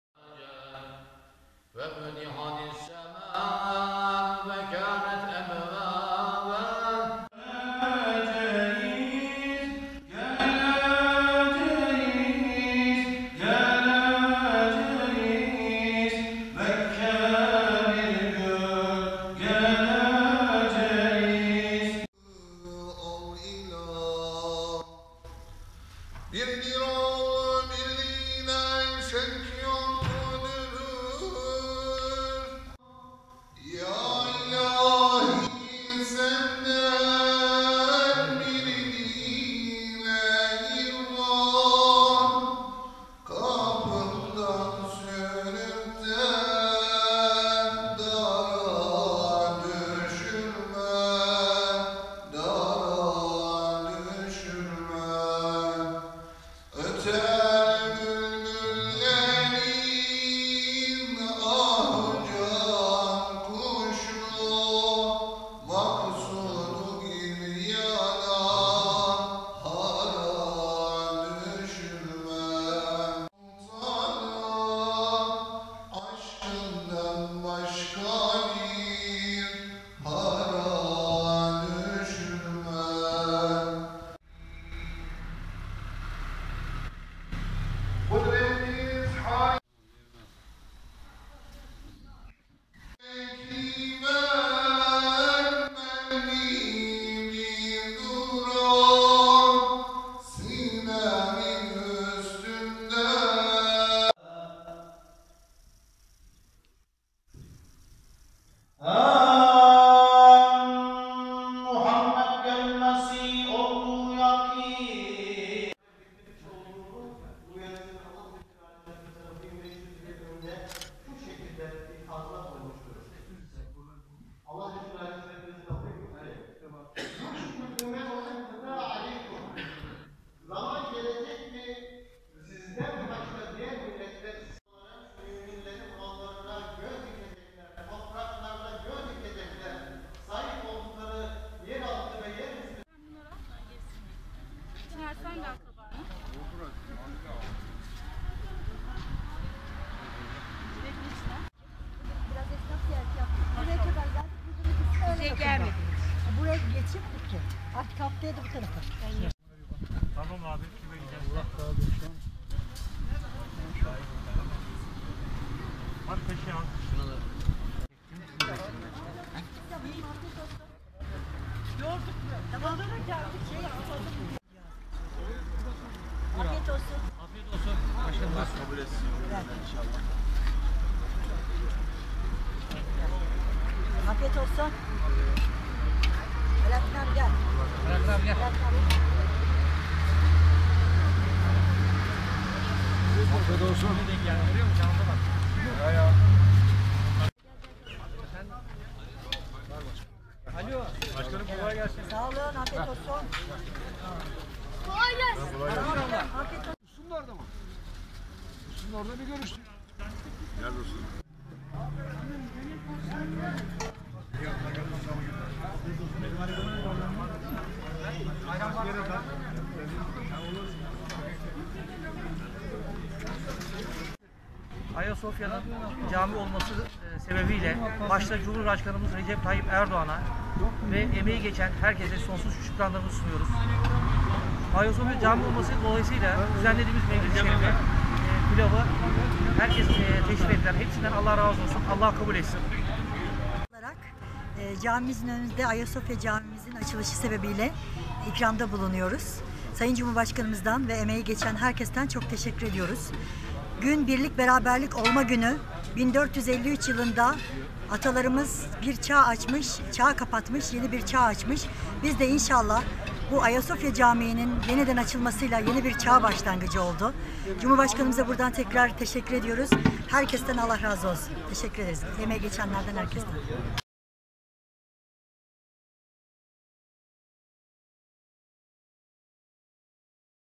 Pazaryeri’nde Ayasofya Camisi'nin ibadete açılması nedeniyle mevlit okundu
Bilecik’in Pazaryeri ilçesinde AK Parti Pazaryeri ilçe Başkanlığınca Ayasofya Camisi'nin 86 yıl sonra cami olarak ibadete açılması dolayısıyla mevlit okutuldu.

Pazaryeri ilçesinde Karamustafa Paşa caminde Cuma namazı öncesi okutulan mevlid-i şerife ilçe halkı büyük katılım sağlarken dualar edildi.

Okutulan Mevlid-i şerifte Sosyal mesafenin korunduğu gözlenirken Cami girişinde Pazaryeri Belediyesi ekipleri mevlite gelen vatandaşlara maske verdi ve dezenfektan sıktı.

Cuma namazı çıkışı AK Parti İlçe Başkanlığınca ilçe halkına Pilav ve ayran ikramı yapılırken ilçe Başkanı İsmail Soydan ve Pazaryeri Belediye Başkanı Zekiye Tekin, vatandaşlara pilav ikram etti.

AK Parti Pazaryeri İlçe Başkanı İsmail Soydan, Ayasofya Camisi'nin 86 yıl sonra cami olarak ibadete açılması nedeni ile Cumhurbaşkanı Recep Tayyip Erdoğan’a teşekkür ederken yaptığı açıklamada, “AK Parti Pazaryeri İlçe Başkanlığı olarak ilçemizde mevlid-i şerif okuttuk. Ayasofya’nın cami olması sebebiyle başta Cumhurbaşkanımız Recep Tayyip Erdoğan’a  ve emeği geçen herkes’e sonsuz şükranlarımı sunuyorum. Ayasofya anın cami olması nedeni ile düzenlediğimiz mevlide herkes teşrif etti herkes den Allah razı olsun Allah kabul etsin” dedi.

Pazaryeri Belediye Başkanı Zekiye Tekin, ise okutulan mevlid’i şerif sonrası, “AK Parti İlçe Başkanlığı olarak Camimizin önünde Ayasofya Camimizin açılışı sebebi ile ikram da bulunuyoruz. Sayın Cumhurbaşkanımızdan ve emeği geçen herkese çok teşekkür ediyoruz. Gün birlik beraberlik olma günü, 1453 yılında atalarımız bir çağ kapatmış yeni bir çağ açmış, bizde inşallah bu Ayasofya camiinin yeniden açılmasıyla yeni bir çağ başlangıcı oldu. Cumhurbaşkanımıza buradan teşekkür ediyoruz herkes ten Allah razı olsun” diye konuştu.

Pazaryeri İlçesinde düzenlenen Mevlid-i şerife Pazaryeri Kaymakamı Berker Çırak, Pazaryeri Belediye Başkanı Zekiye Tekin, AK Parti Pazaryeri ilçe Teşkilatı İl Genel Meclis üyeleri Alattin Emral, Mehmet Pulat, Belediye meclis üyeleri ve İlçe halkı katıldı.